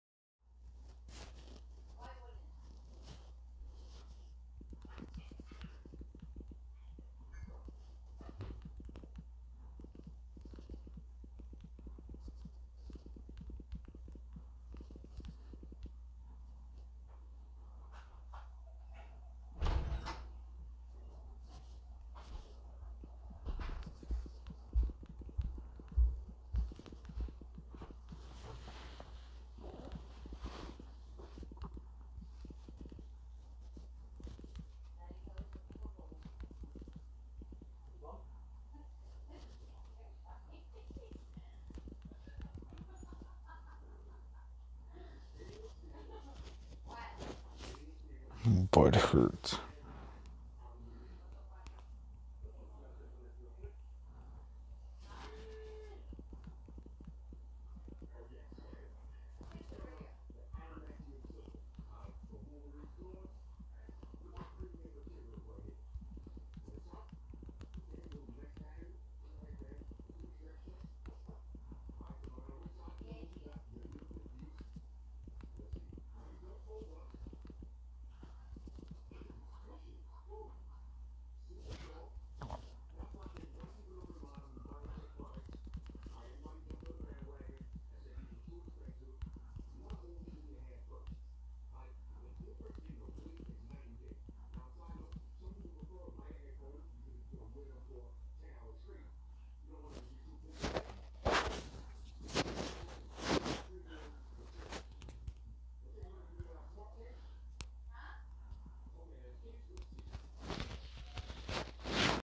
Can't comment on YouTube lol
Endgay fanboys probably that angry that they probably riot all over Google just to make it #1 again lol. It's been 6 months and they still mad about it lol